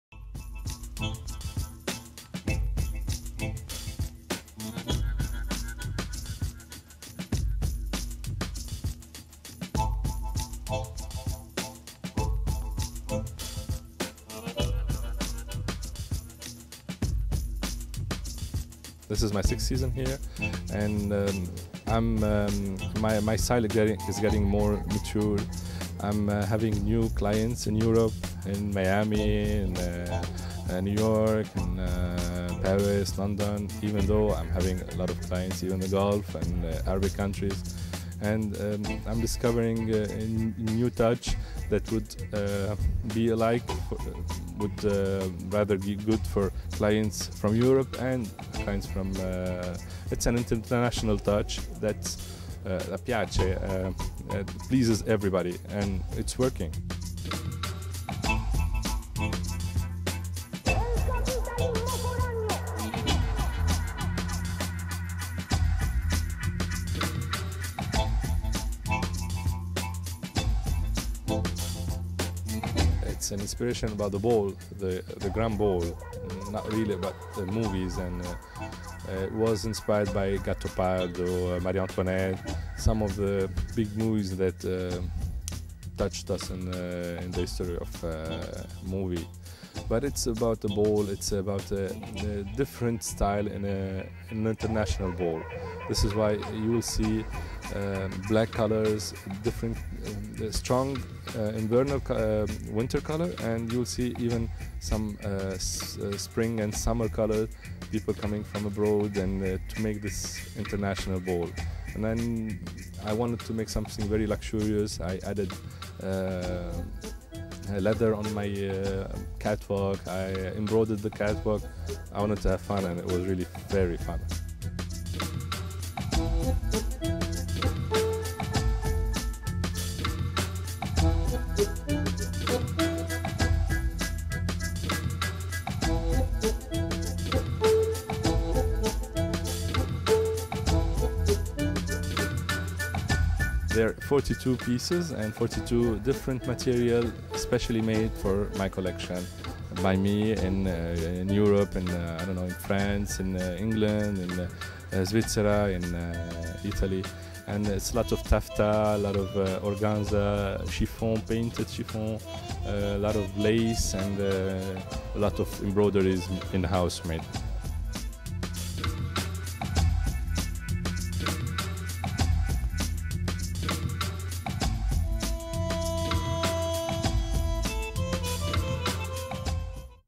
tony ward hair & makeup backstage alta moda roma f/w 06/07
Your backstage pass to the latest hair and make-up trends; including beauty tips on how to do your own make-up to make sure you get noticed.